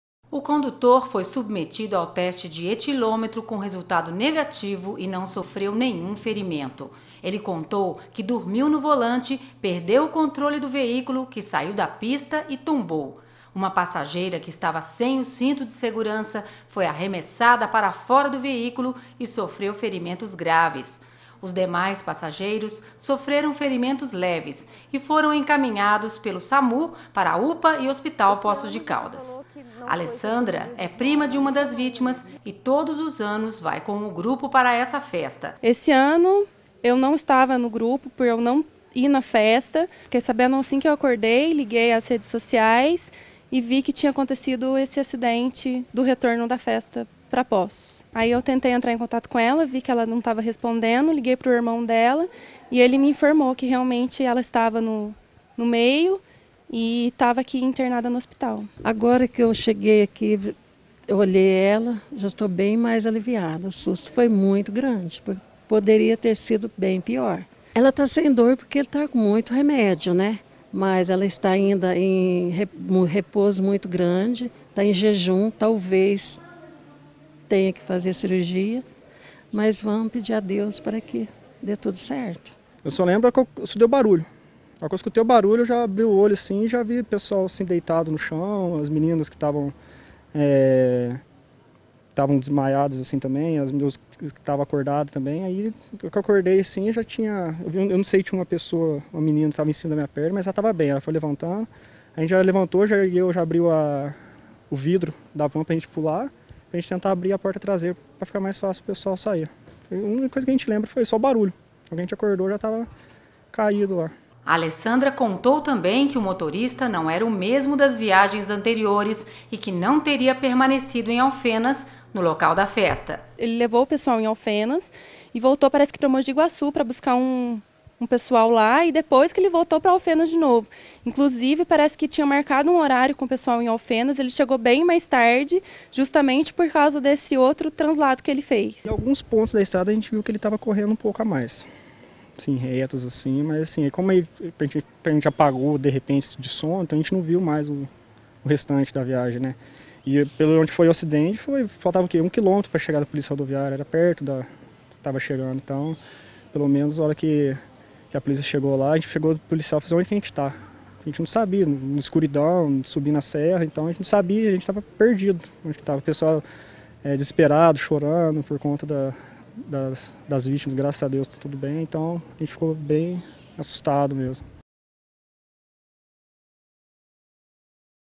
ACIDENTE VAN JOVENS
Ocorreu hoje em Poços de Caldas, por volta das quatro e quinze da madrugada deste domingo (07), um acidente na BR146, km 516,3. O acidente envolveu o veículo Mercedes Benz Sprinter com placas de Poços, que transportava 15 jovens. Eles retornavam de uma festa em Alfenas. O condutor, que não sofreu nenhum ferimento, contou que dormiu no volante, perdeu o controle do veículo, que saiu da pista e tombou.